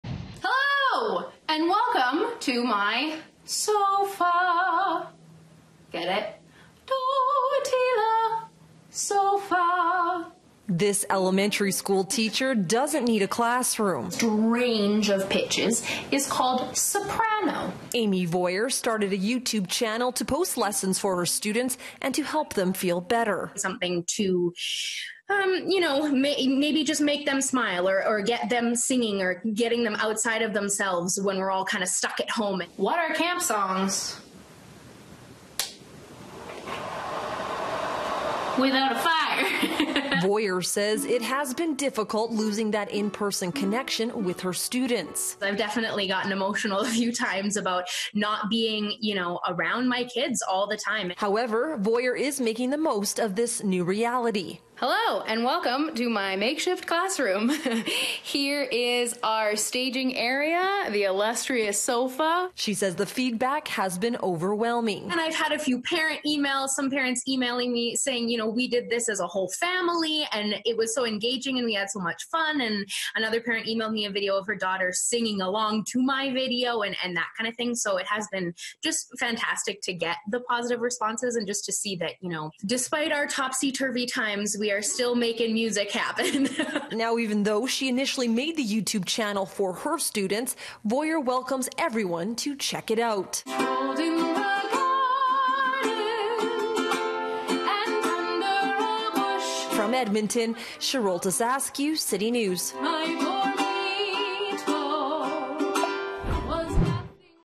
Edmonton music teacher launches educational YouTube channel
From the classroom, to YouTube. An Edmonton music teacher has launched an educational channel for her students. Sarolta Saskiw has her story.